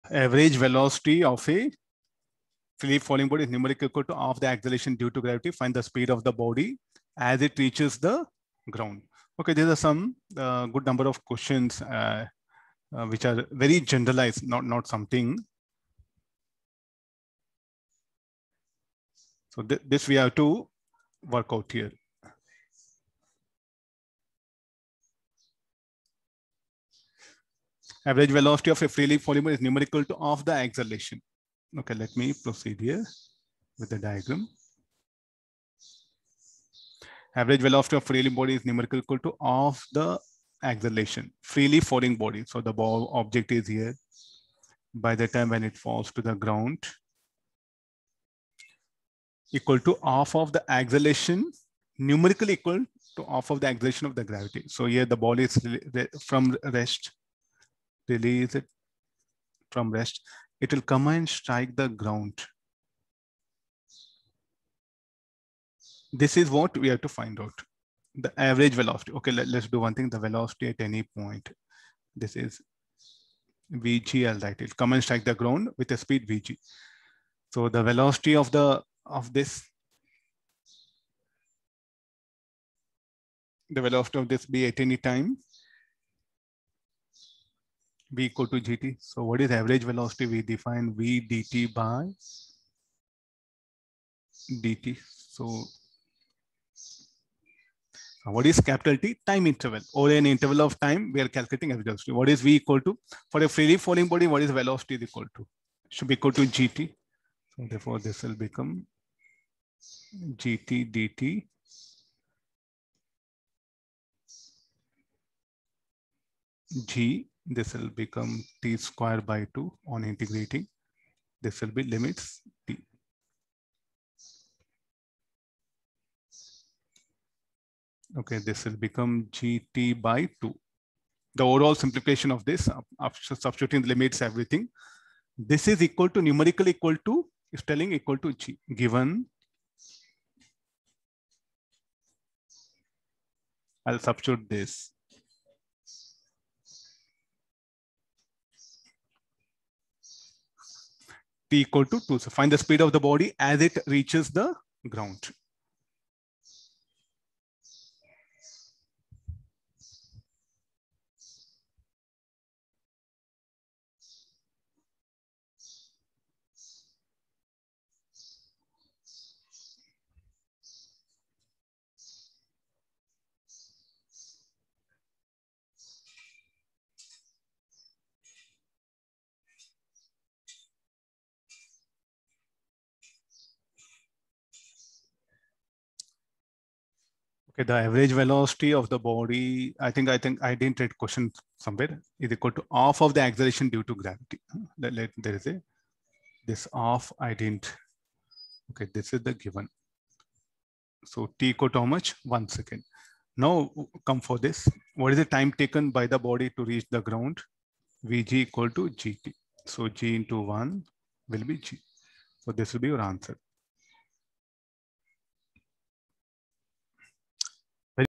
26) JEE | The average velocity of a freely falling body is numerically equal to half of the
acceleration due to gravity. The velocity of the body as it reaches the ground is numerically equal to: | Physics | Kinematics | Motion under gravity class 11 |